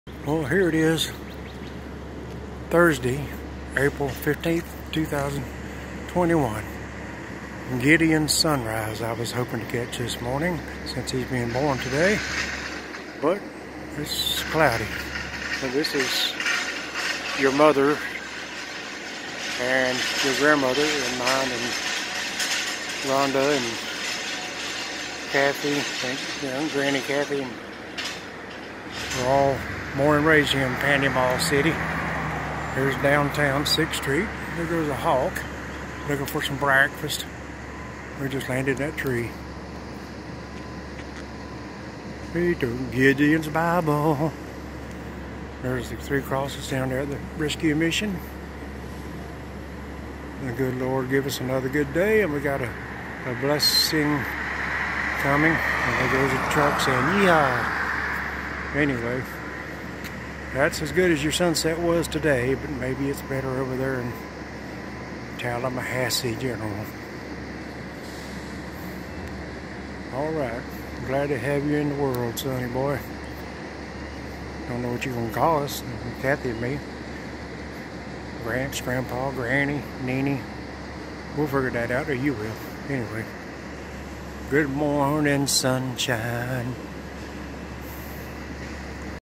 Gideon's first Sunrise almost or a facsimile thereof part 1
you are actually being born this afternoon so this is not really good first Sunrise. That will be tomorrow morning which maybe will be brighter if I don't forget to do it.